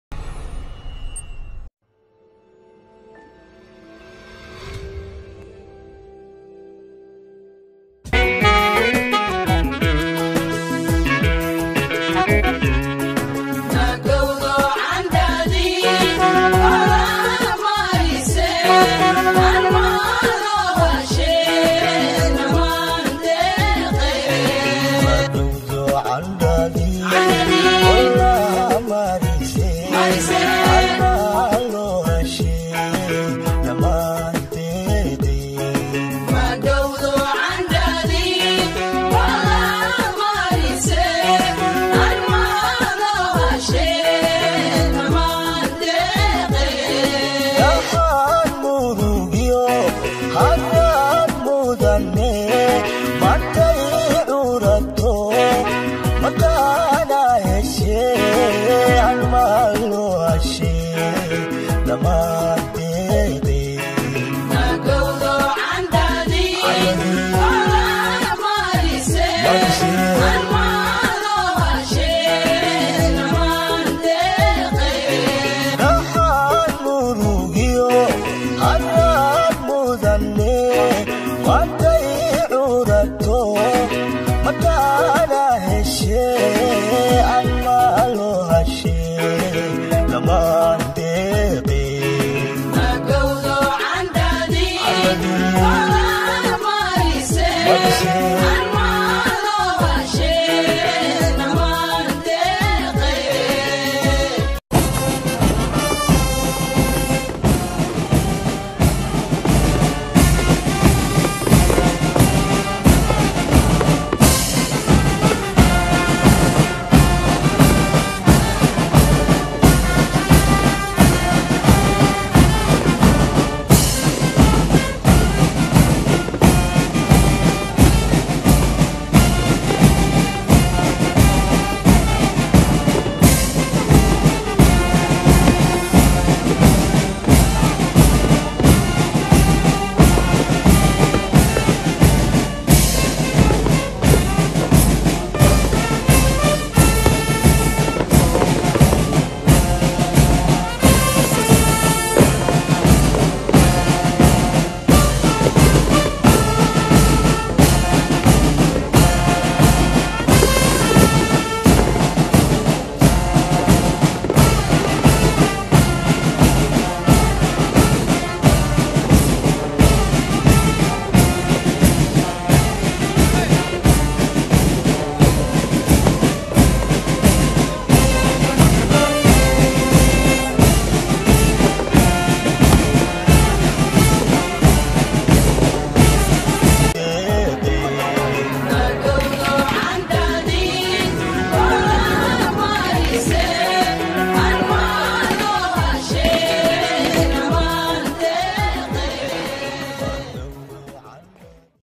Dabaaldagii kowda luuliyo iyo Bandhigyo qurux badan || Gobey production
61 sano guuradii kasoo wareegatay xurnimadii soomalia iyo midowgii labadii gobol woqooyi iyo koonfur

soomalia hanoolaato || Somalia ha israacdo